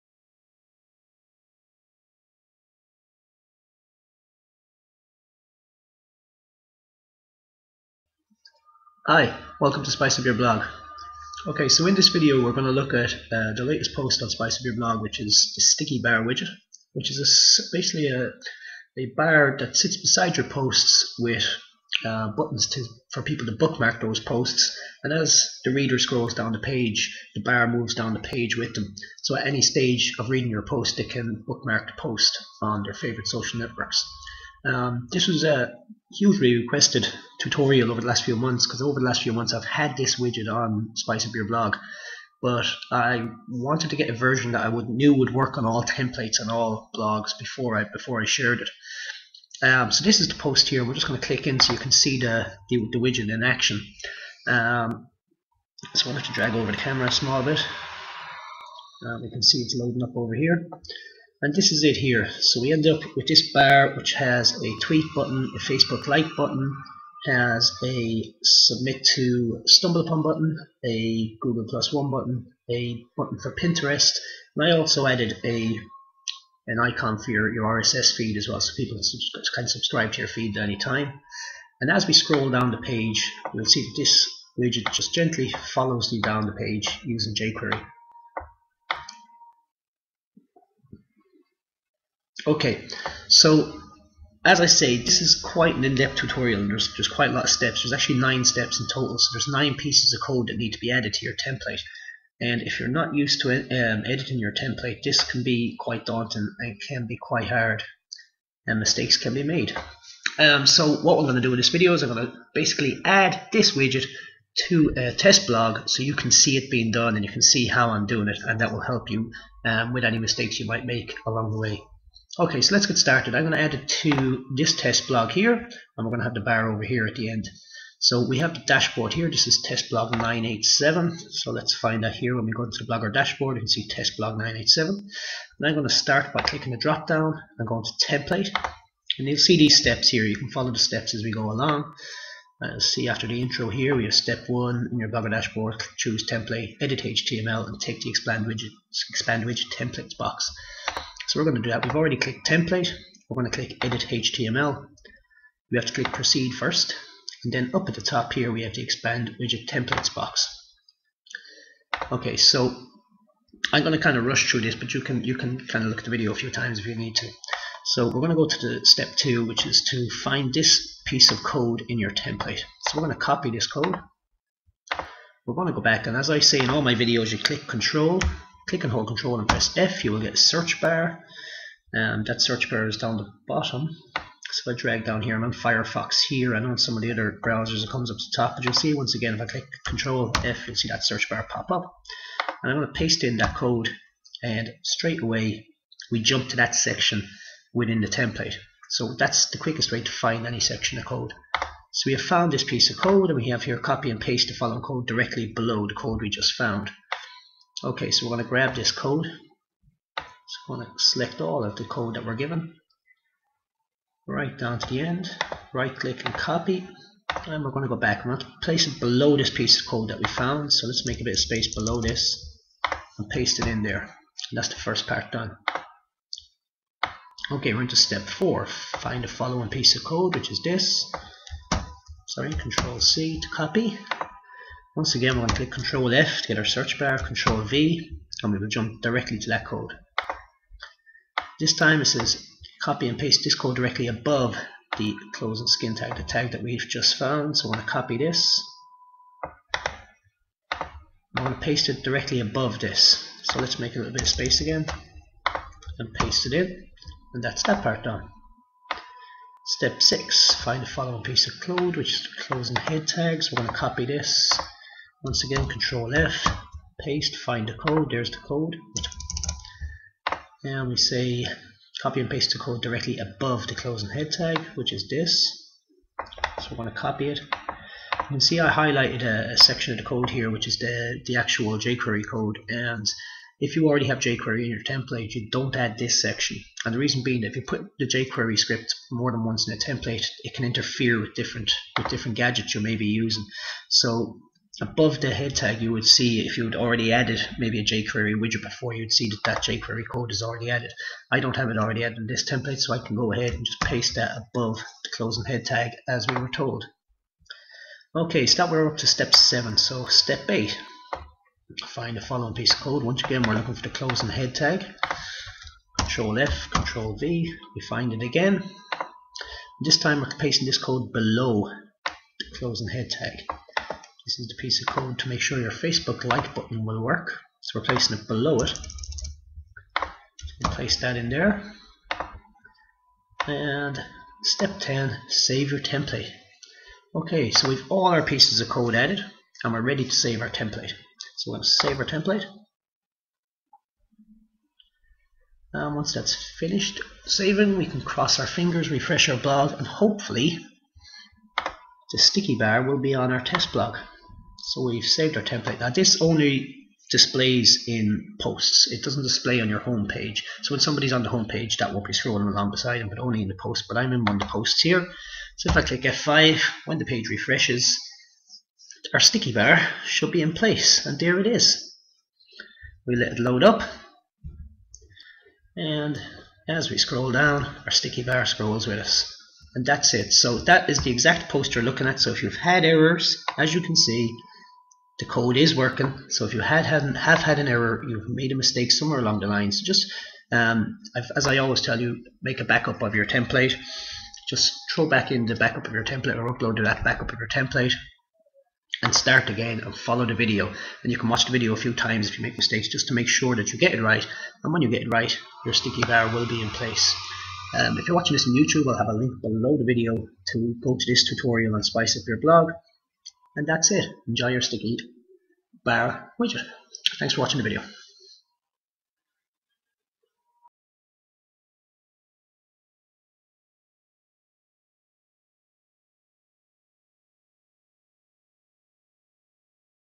Sticky Bar - Fixed Slide Social Bookmark Bar For Blogger
This jQuery bar sits beside your posts with buttons for Twitter, Facebook and other social networks.As the reader scrolls the page the bar follows.